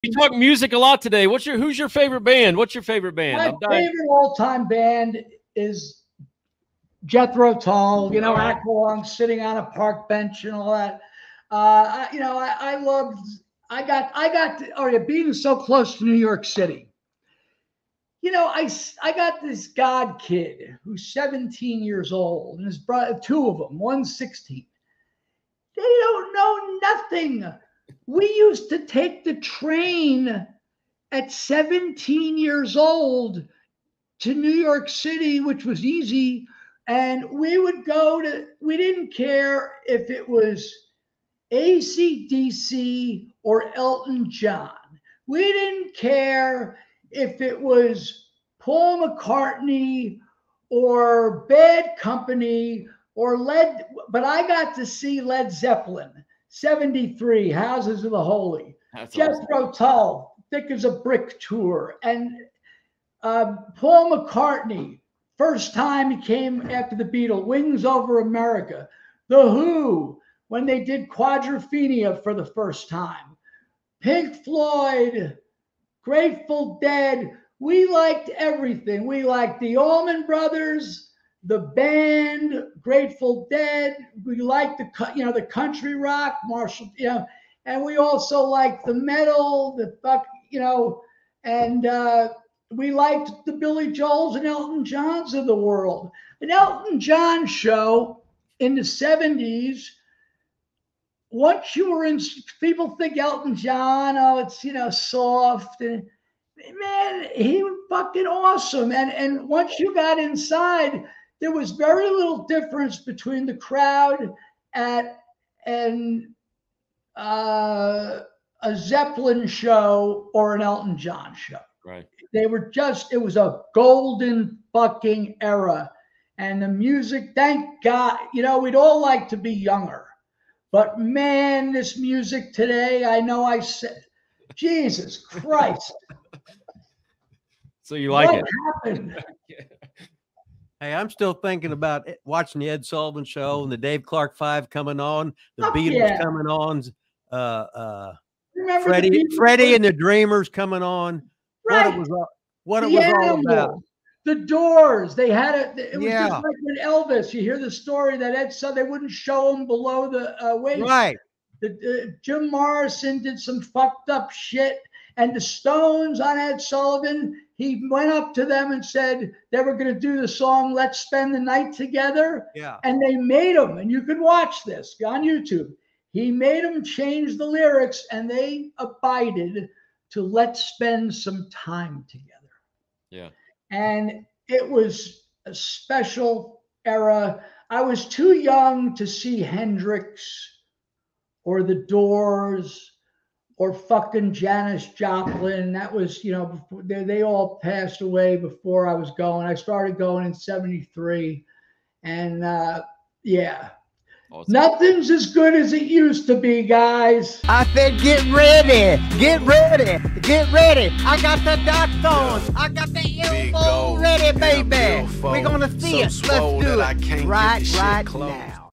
Rip Rogers and Cary Silkin talk music!
Cary Silkin, the former Ring of Honor owner, talks music with Wrestling with Rip Rogers! Silkin likes everything from Led Zepplin to Elton John, but his favorite band of all time is Jethro Tull!

Rip Rogers Book on Pro Wrestling (Amazon)